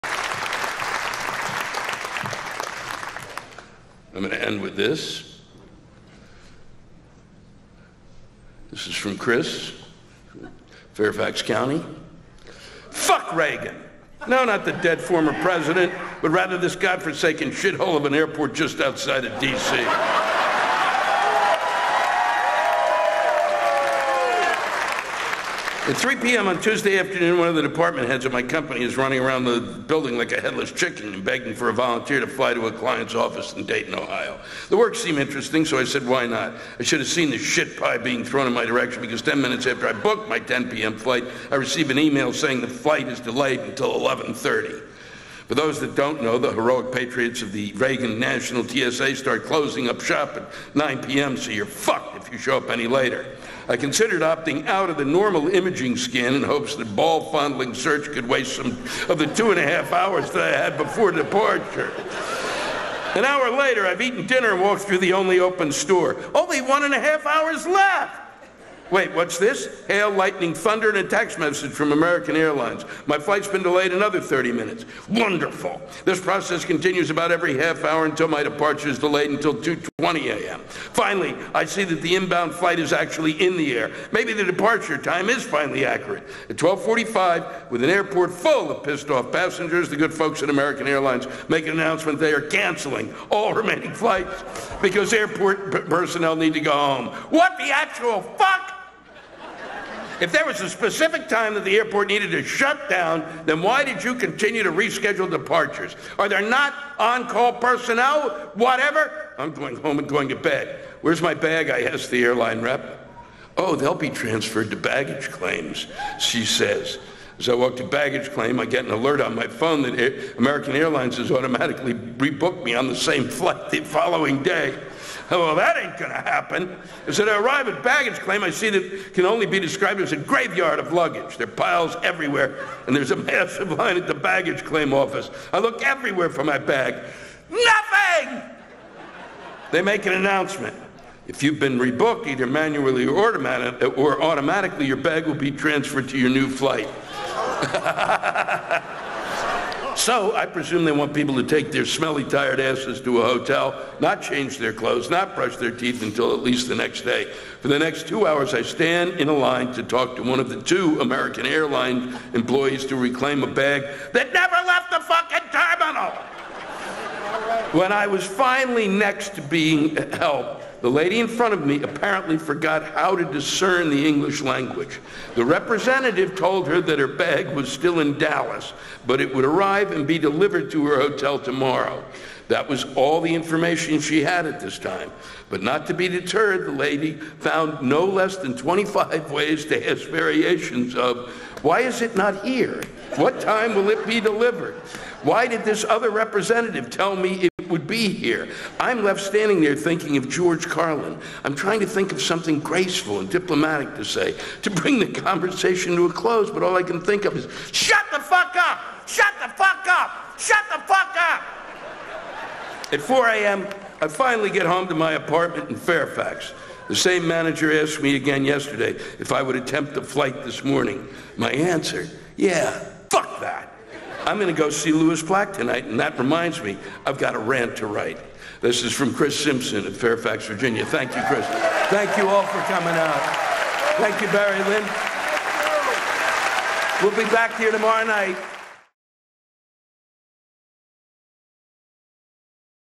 Lewis Black @ Warner Theatre on May 17, 2018
Lewis Black's end-of-set listener rant session live from Washington, DC on May 17, 2018